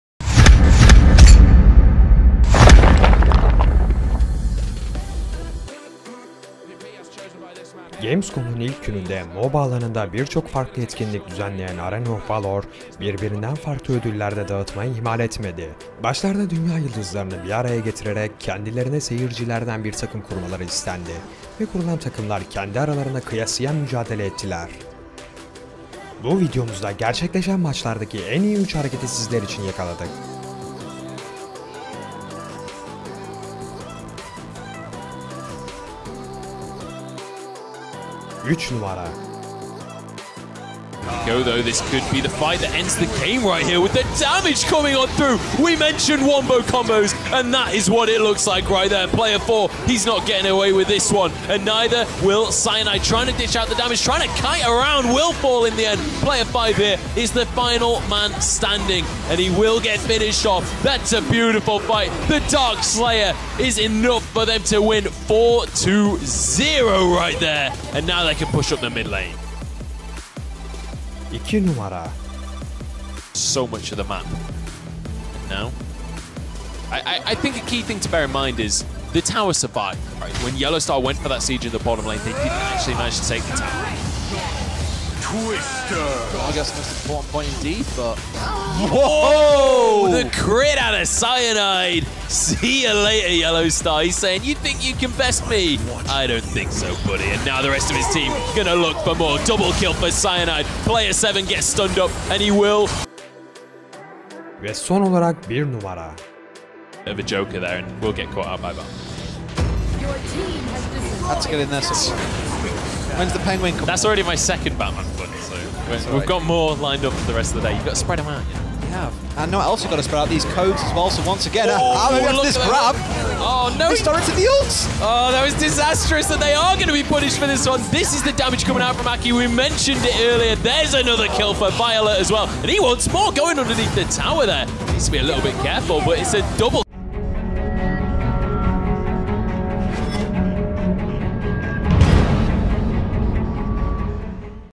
Arena of Valor Gamescom 2017 1. Gün - İlk 3 Hareket
Gün içerisinde Almanya Köln’de gerçekleşen Gamescom oyun fuarında birbirinden etkileyici şovlara tanıklık ettik. Özellikle yıllardır MOBA alanında az etkileşim alan Gamescom, bu yıl eşsiz bir yapılanma ile müthiş bir katılım aldı. Tabii ki MOBA alanının gözdesi Tencent Gaming tarafından geliştirilen Arena of Valor oldu.